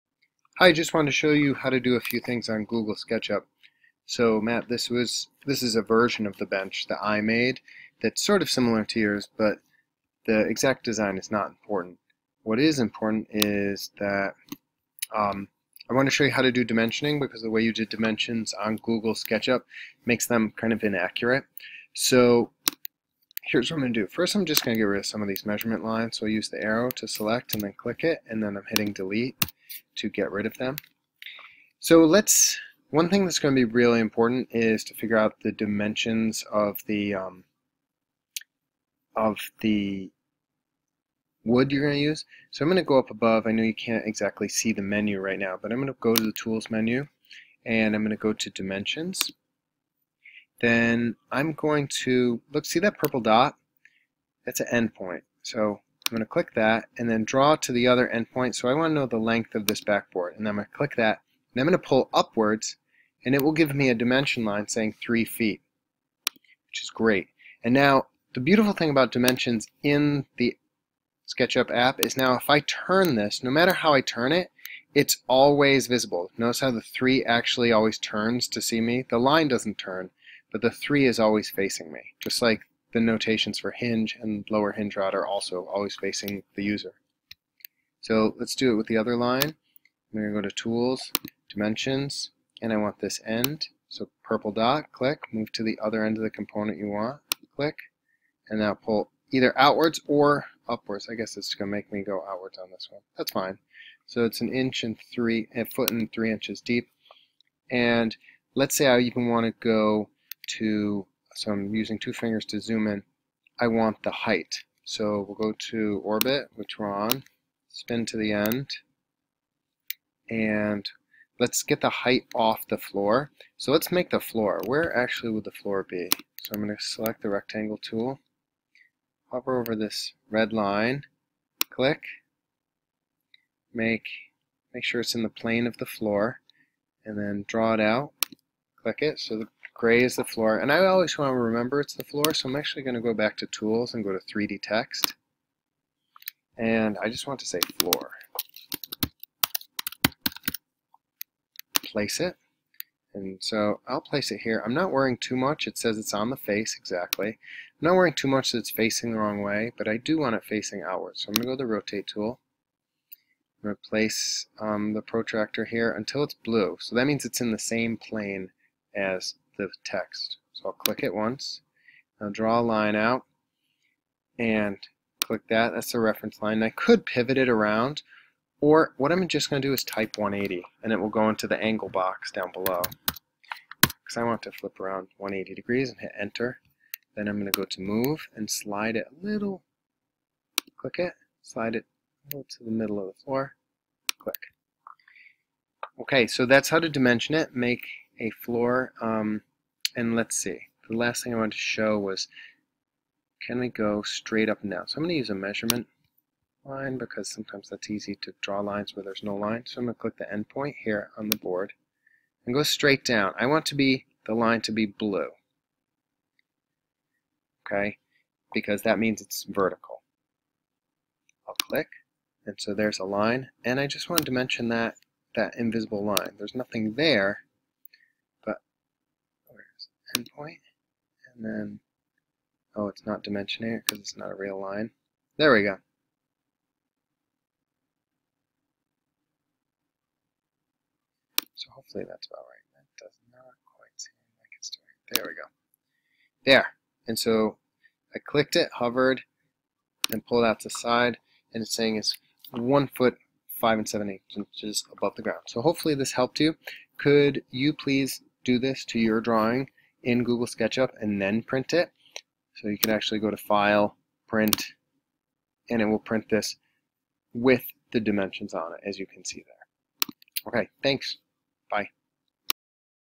Adding dimensions in sketchup
A short video on how to add dimensions, a floor, and 3D text to a sketch.